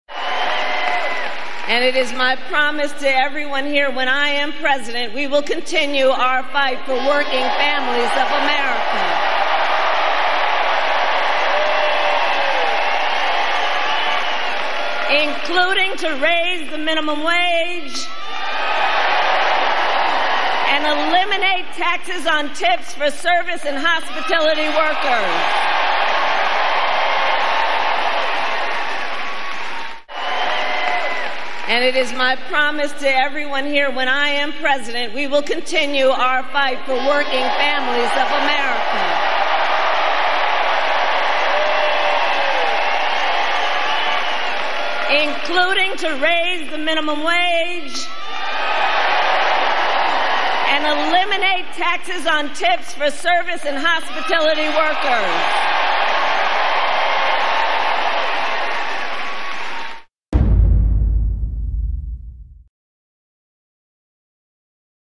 Harris ‘copies’ Trump’s no tax on tip policy at rally in hospitality-heavy Nevada
Vice President Kamala Harris rolled out a promise to stomp out taxes on tips during a campaign stop in Las Vegas Saturday — prompting former President Donald Trump to rip her for “copying” one of his signature proposals.

Though she spent a significant portion of her talk ripping Trump, the Democratic presidential nominee regurgitated one of his signature ideas at the top of her stump speech at the battleground state.

“It is my promise to everyone here, when I am President, we will continue our fight for working families of America, including to raise the minimum wage and eliminate taxes on tips for service and hospitality workers,” Harris told the crowd to a roar of applause.